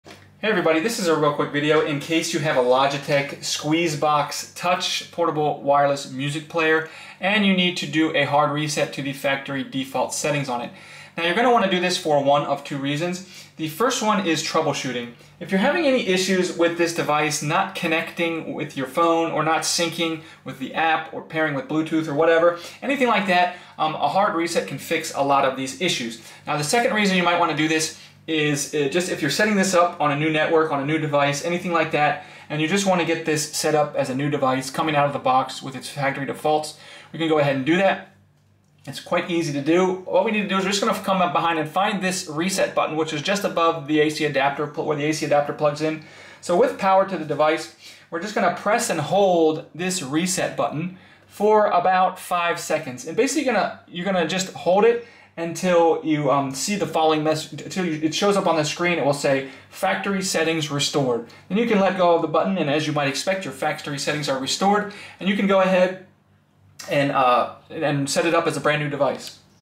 Hard Reset Logitech Squeezebox Touch
How to troubleshoot or setup starting with a force hard reset a Logitech Squeezebox Touch to factory default settings if it is not working or not connecting or if it is frozen or not responding, the factory reset is a good troubleshooting step. You can also restore the factory settings to setup your device as new. Found in the Logitech Squeezebox Touch user manual.
Time for a new Bluetooth speaker?